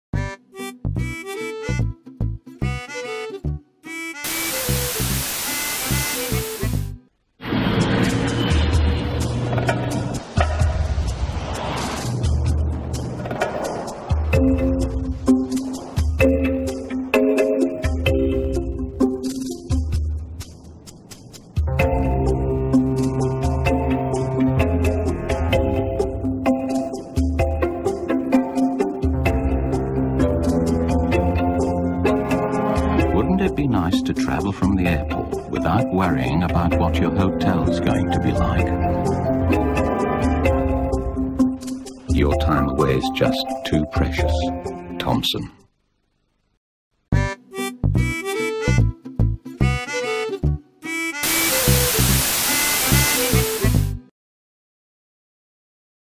Thomson Advert - 2004
An advert for Thomson  shown on Central (now ITV) in 2004.